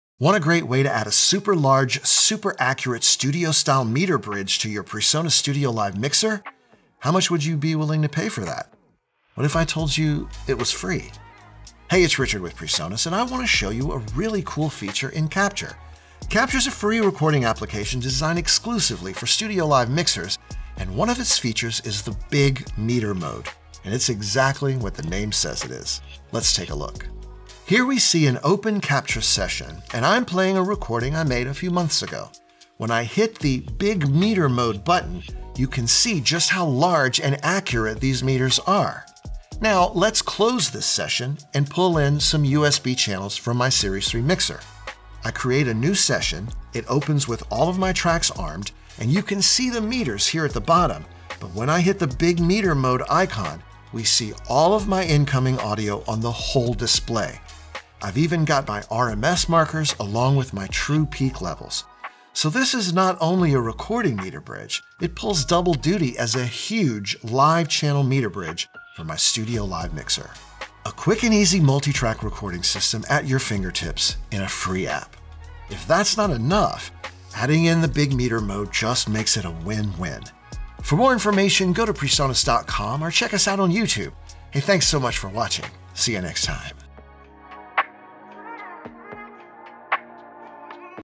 The Big Meter Mode in PreSonus Capture
Looking to add a large, accurate meter bridge to your PreSonus StudioLive mixer? In this StudioLive Snapshot, PreSonus product expert Richard Gaspard demonstrates what this looks like, and how easy it is to set up with PreSonus Capture.